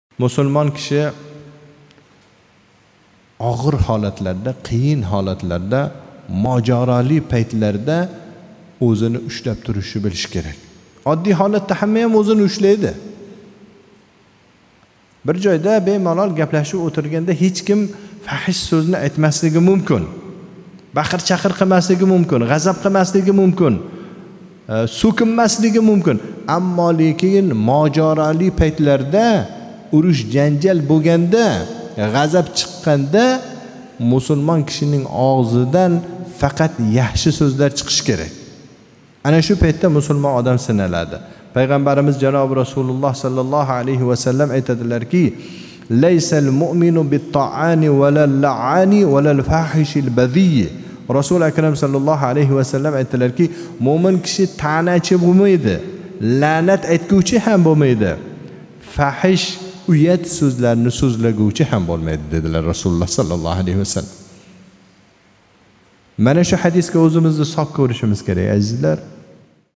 Мўмин киши фахш сўзларни сўзлагувчи бўлмайди@Shariatuz
Мўмин киши фаҳш сўзларни сўзлагувчи бўлмайди

Ҳасан домла Қодиров
Сирож Cолиҳ жоме масжиди имом-хатиби